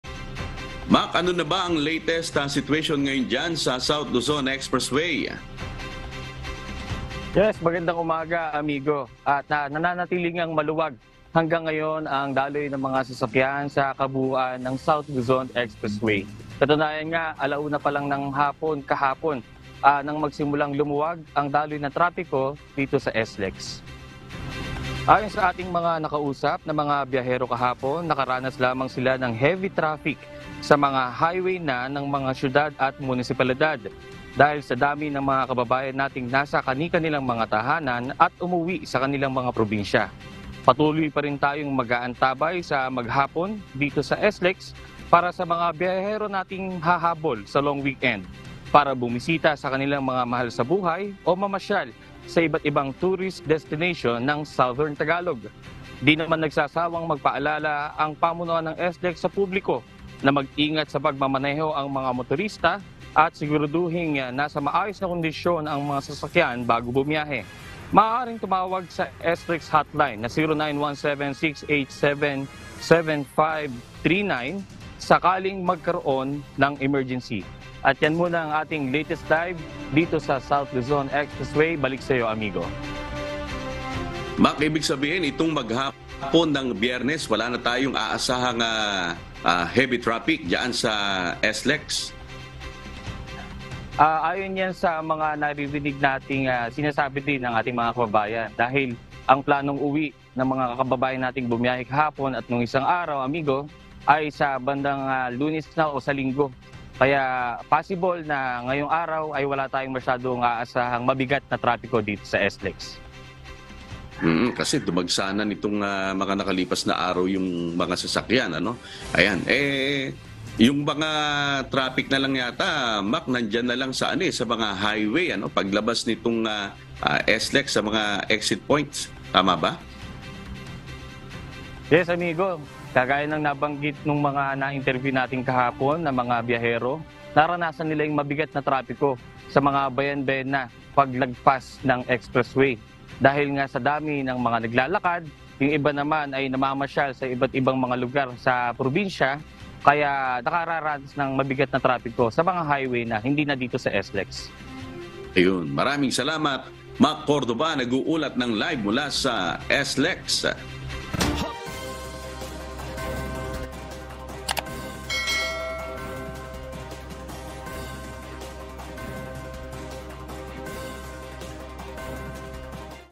SLEX Alabang to Sto. Tomas Exit, vice versa maluwag pa rin
Maghapong maluwag na biyahe ang maaaring asahan ng mga biyahero sa South Luzon Expressway ngayong araw.